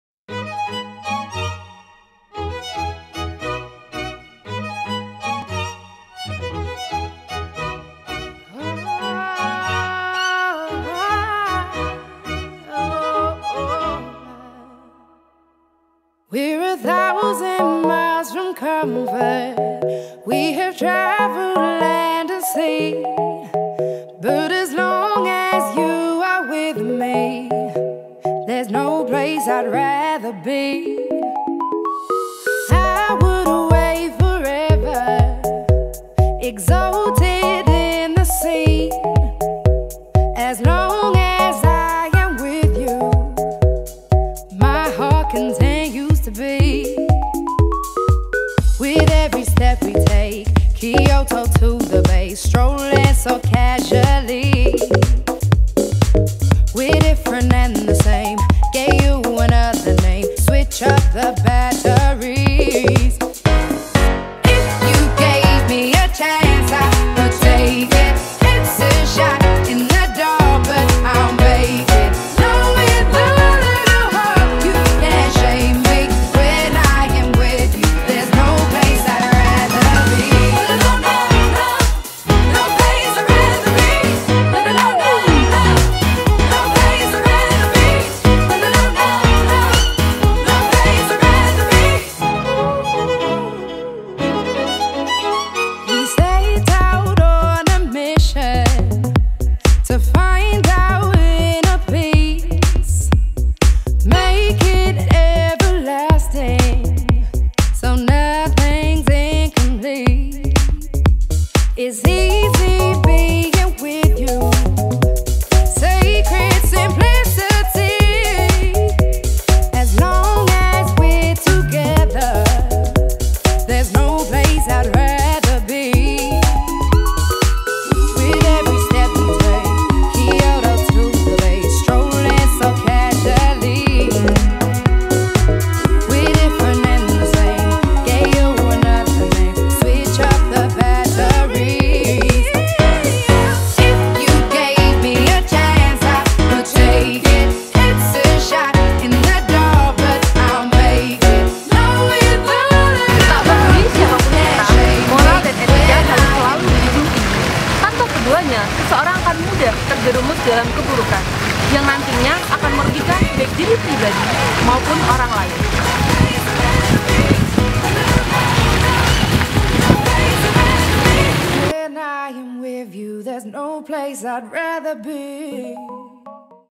Etika Terhadap Karyawan Kampus-EkonomiTeknik15
Etika Terhadap Karyawan Kampus by Team 15 (Ekonomi Teknik)

Sebagai seorang mahasiswa, etika setiap pribadi perlu diterapkan dalam seluruh lapisan anggota masyarakat kampus. Termasuk karyawan kampus yang patut harus kita hargai jasa-jasa mereka.